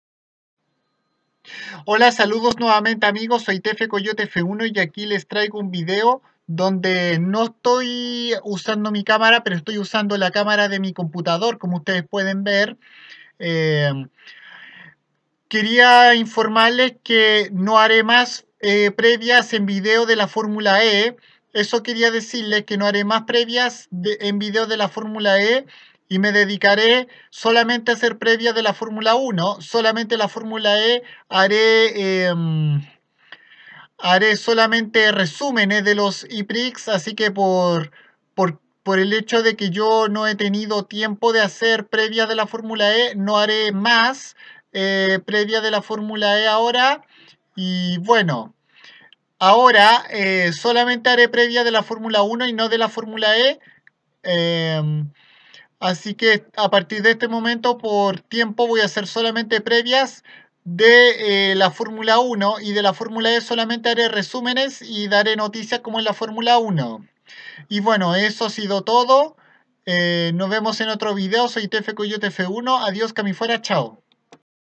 Vídeo informativo de TFcoyoteF1
En este vídeo informo que ya no haré mas previas de la Formula E y que solo haré resúmenes de la categoría y daré noticias de la misma haré previas solo de la F1

Vídeo grabado desde mi PC